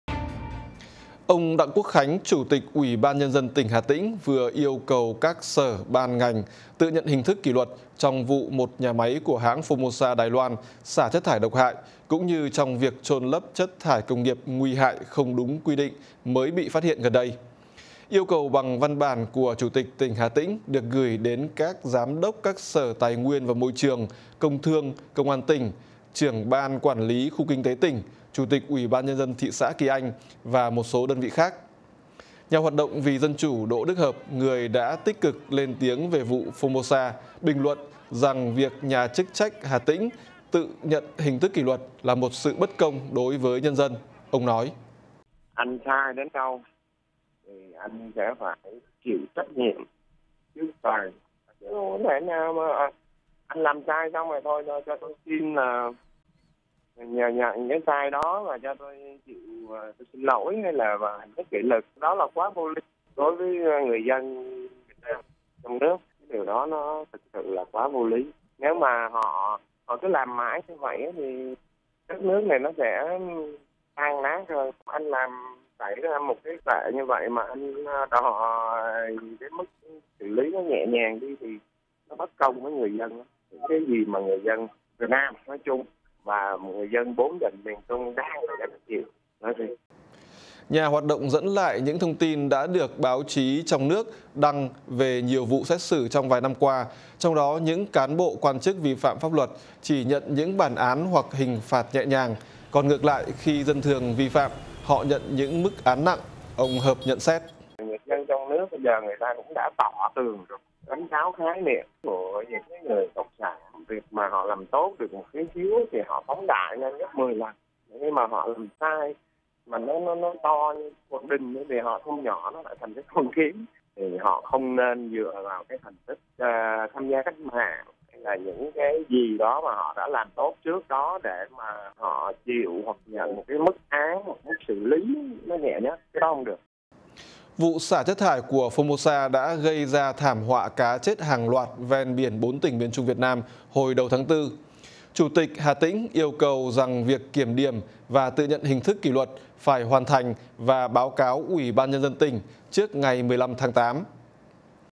Hà Tĩnh 'tự nhận hình thức kỷ luật' trong vụ Formosa là 'bất công với dân'
Tin tức Nếu không vào được VOA, xin các bạn hãy vào để vượt tường lửa. Báo chí Việt Nam hôm 4/8 đưa tin ông Đặng Quốc Khánh, Chủ tịch Ủy ban Nhân dân tỉnh Hà Tĩnh vừa yêu cầu các sở, ban, ngành tự nhận hình thức kỷ luật trong vụ một nhà máy của hãng Formosa, Đài Loan, xả chất thải độc hại, gây ra thảm họa môi trường biển hồi đầu tháng 4.

Yêu cầu bằng văn bản của vị chủ tịch tỉnh Hà Tĩnh được gửi đến các giám đốc các sở Tài nguyên và Môi trường, Công thương, công an tỉnh, trưởng Ban quản lý khu kinh tế tỉnh, chủ tịch UBND thị xã Kỳ Anh, và một số đơn vị khác.

Chủ tịch tỉnh yêu cầu họ phải “tổ chức tự kiểm điểm và chỉ đạo kiểm điểm nghiêm túc trách nhiệm của tập thể, cá nhân có liên quan” trong việc nhà máy của Formosa xả thải, gây hại cho môi trường, cũng như trong việc chôn lấp chất thải công nghiệp nguy hại không đúng quy định bị phát hiện gần đây. Văn bản của ông chủ tịch cũng yêu cầu các cơ quan kê trên “tự nhận hình thức kỷ luật và có hình thức kỷ luật nghiêm theo quy định của pháp luật”.

Từ thành phố Hồ Chí Minh, nhà hoạt động vì dân chủ Đỗ Đức Hợp, người đã tích cực lên tiếng về vụ Formosa, bình luận với VOA rằng việc nhà chức trách Hà Tĩnh sẽ “tự nhận hình thức kỷ luật” là một “sự bất công” đối với nhân dân. Ông Hợp nói rõ hơn:

“Anh sai đến đâu thì anh sẽ phải chịu trách nhiệm trước tòa. Chứ không có thể nào mà anh làm sai xong nói thôi cho tôi xin là nhận cái sai đó và cho tôi xin lỗi và nhận hình thức kỷ luật. Đó là quá vô lý. Đối với người dân Việt Nam, trong nước, điều đó nó thực sự là quá vô lý. Nếu mà họ cứ làm mãi như vậy thì đất nước này sẽ tan nát hơn. Anh làm xảy ra một cái tệ như vậy mà anh đòi cái mức xử lý nó nhẹ nhàng đi thì nó bất công với người dân lắm, những cái gì mà người dân Việt Nam nói chung và người dân 4 tỉnh miền trung đang bị gánh chịu nói riêng”.

Nhà hoạt động Đỗ Đức Hợp dẫn lại những thông tin đã được báo chí trong nước đăng về nhiều vụ xét xử trong vài năm qua trong đó những cán bộ, quan chức vi phạm pháp luật chỉ nhận những bản án hoặc hình phạt nhẹ nhàng, còn ngược lại, khi dân thường vi phạm, họ nhận những mức án nặng. Ông Hợp nhận xét:

“Người dân trong nước bây giờ người ta cũng tỏ tường rồi. Đánh tráo khái niệm của người cộng sản. Việc mà họ làm tốt được một tí xíu thì họ phóng đại lên gấp 10 lần. Nhưng mà họ làm sai, nếu nó to như cái cột đình thì họ thu nhỏ lại bằng cái con kiến. Họ không nên dựa vào cái thành tích tham gia cách mạng hoặc cái gì đó họ đã làm tốt trước đó để họ chịu hoặc nhận các cái mức án hoặc mức xử lý nhẹ nhất. Cái đó không được”.

Vụ xả chất thải của một nhà máy sắp vận hành thuộc Formosa đã gây ra thảm họa cá chết hàng loạt ven biển 4 tỉnh miền trung Việt Nam hồi đầu tháng 4.

Chính văn bản của chủ tịch tỉnh Hà Tĩnh yêu cầu cấp dưới kiểm điểm và tự nhận hình thức kỷ luật cũng nêu rõ “vụ việc Công ty TNHH Gang thép Hưng nghiệp Formosa Hà Tĩnh xả thải, gây ra sự cố môi trường biển và một số doanh nghiệp, tổ chức, cá nhân chôn lấp chất thải rắn, đổ rác thải không đúng quy định trên địa bàn thị xã Kỳ Anh trong thời gian qua, đã gây hậu quả nghiêm trọng, ảnh hưởng đến kinh tế - xã hội và gây bức xúc trong quần chúng nhân dân, làm ảnh hưởng đến an ninh trật tự và kế hoạch phát triển kinh tế - xã hội của tỉnh”.

Ông Khánh, chủ tịch tỉnh, yêu cầu rằng việc kiểm điểm và tự nhận hình thức kỷ luật phải “hoàn thành và báo cáo UBND tỉnh trước ngày 15/8”.